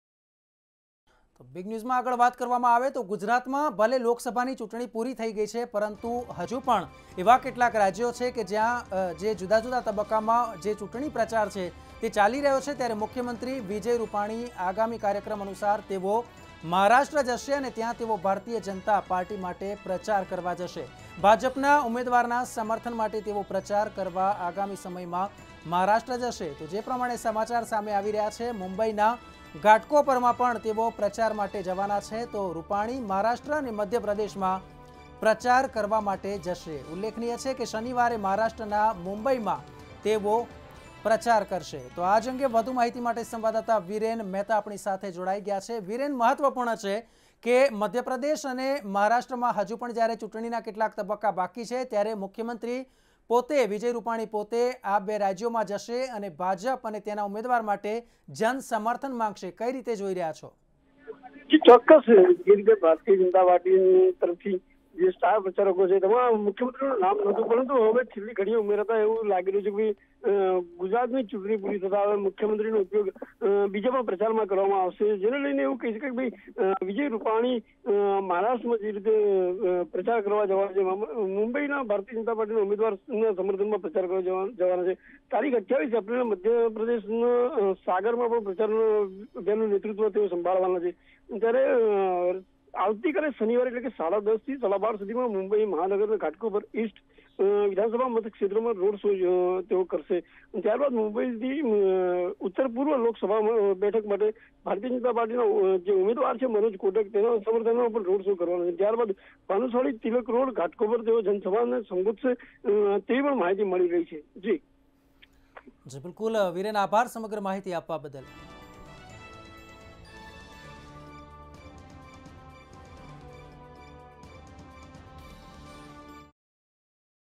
CM Vijay Rupani મહારાષ્ટ્ર અને મધ્યપ્રદેશમાં કરશે પ્રચાર - Mantavya News
CM Vijay Rupani will campaign in Maharashtra and Madhya Pradesh